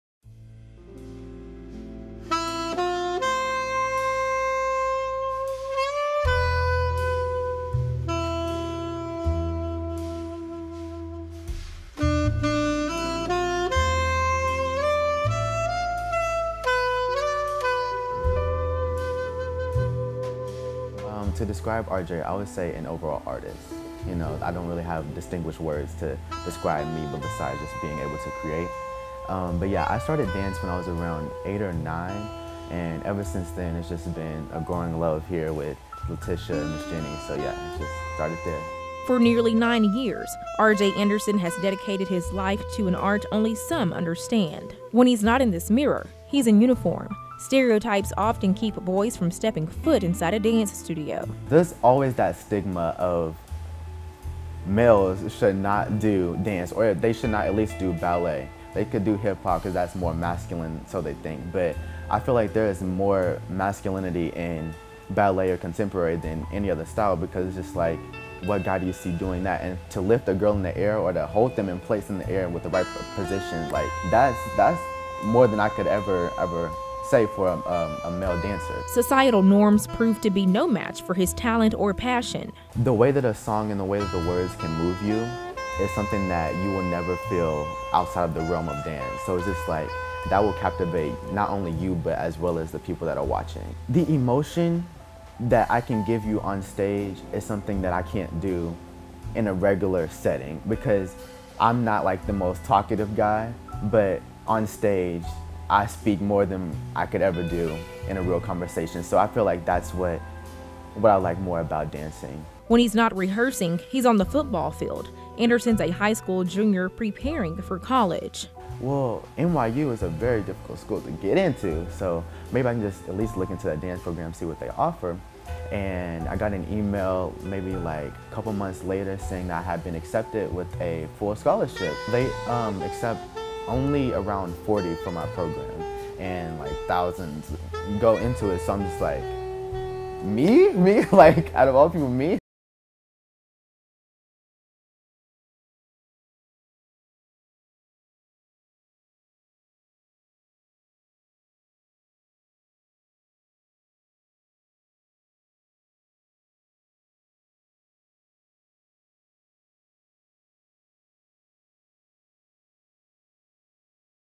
High school football player dances his way to NYU scholarship
RJ Anderson, a junior and football player at Pulaski Academy, is not your average dancer and certainly not your average athlete. He's heading to New York this summer for a program thousands apply for, but only few attend.